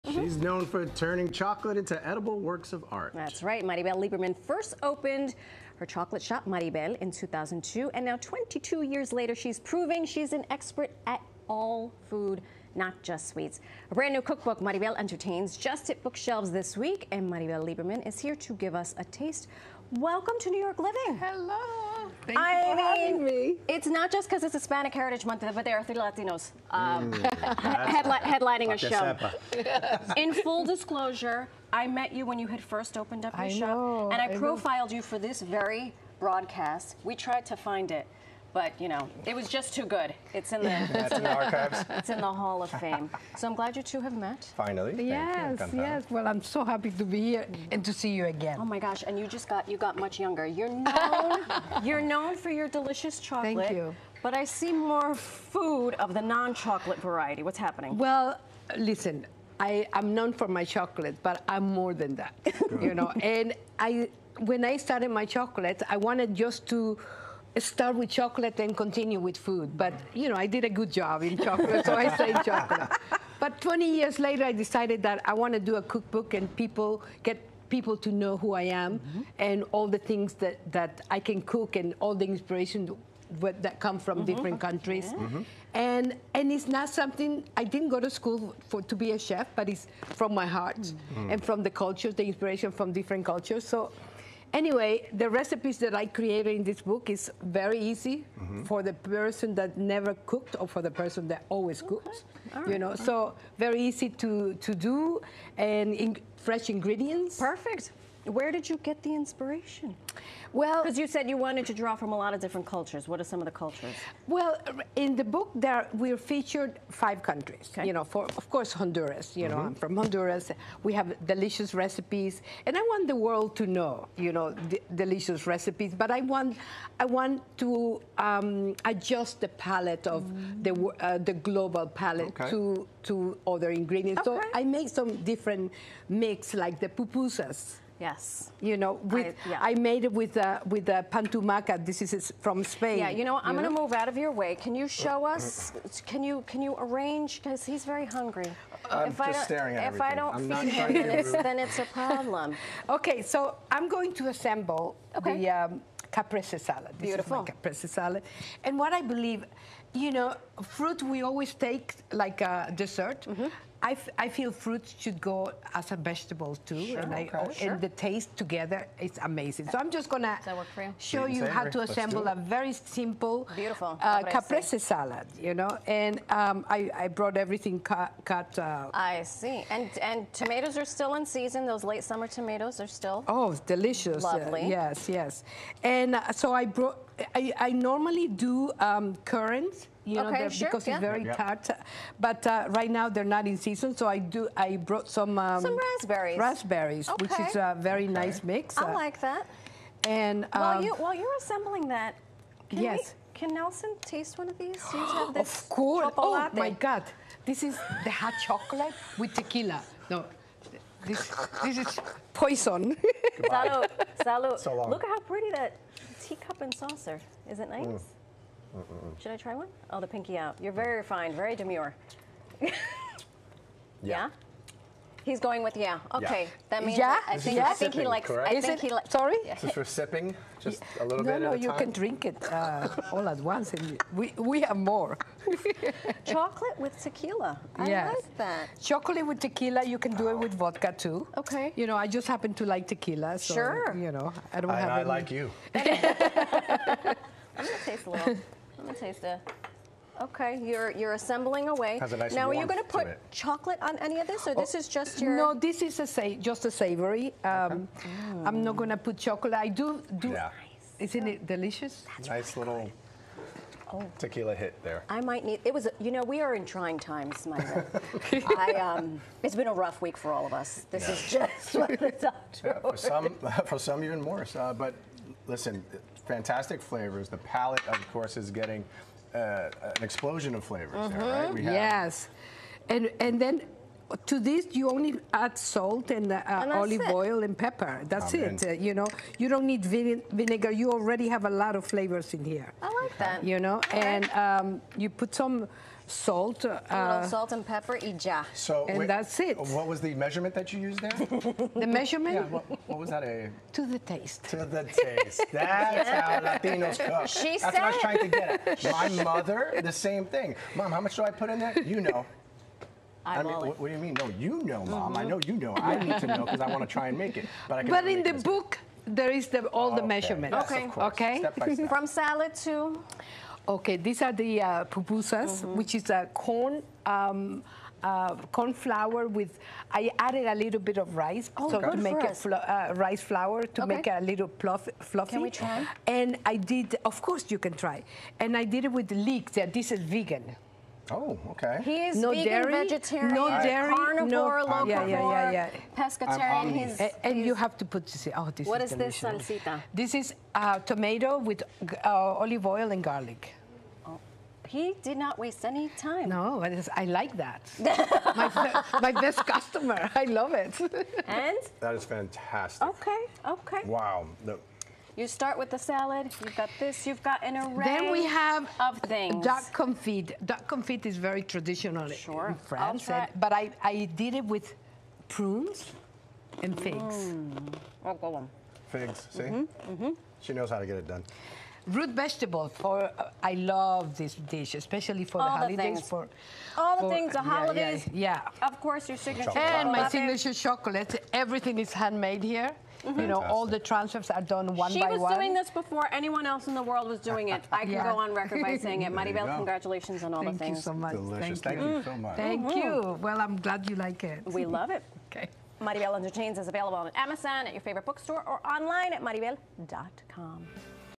Make edible art with the 'Mariebelle Entertains' cookbook
Known for making edible art with chocolate, Maribel Lieberman reveals her baking secrets with her new cookbook "Mariebelle Entertains."